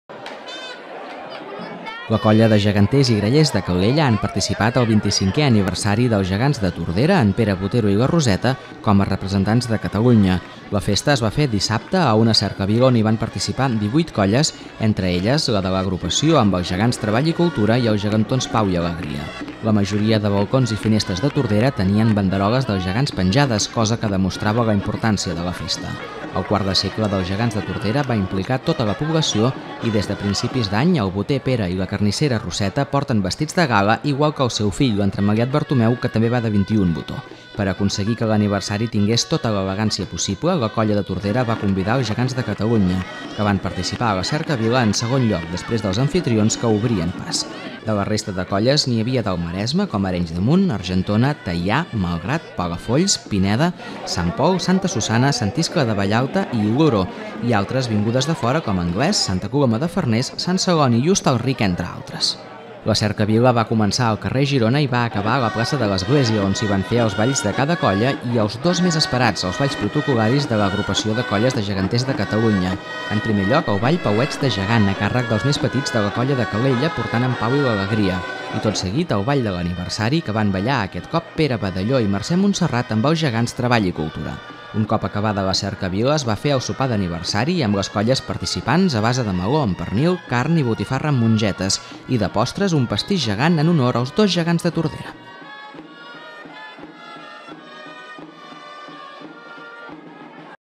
Calella porta en Treball i la Cultura a l'aniversari dels gegants de Tordera
La colla de Geganters i Grallers de Calella han participat al 25è aniversari dels Gegants de Tordera, en Pere Botero i la Roseta, com a representants de Catalunya. La festa es va fer dissabte, amb una cercavila on hi van participar 18 colles, entre elles la de l'Agrupació, amb els Gegants Treball i Cultura, i els gegantons Pau i Alegria.